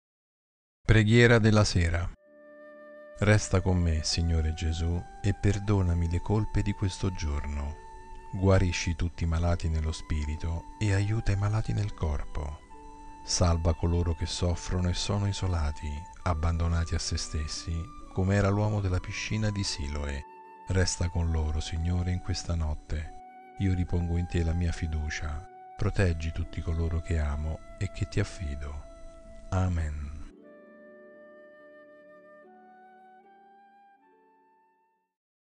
Preghiera della sera 2 Aprile 2021
Resta con me, Signore Gesù, e perdonami le colpe di questo giorno. Guarisci tutti i malati nello spirito e aiuta i malati nel corpo. Salva coloro che soffrono e sono isolati, abbandonati a se stessi come era l'uomo della piscina di Siloe. Resta con loro, Signore, in questa notte; io ripongo in te la mia fiducia. Proteggi tutti coloro che amo e che ti affido. Amen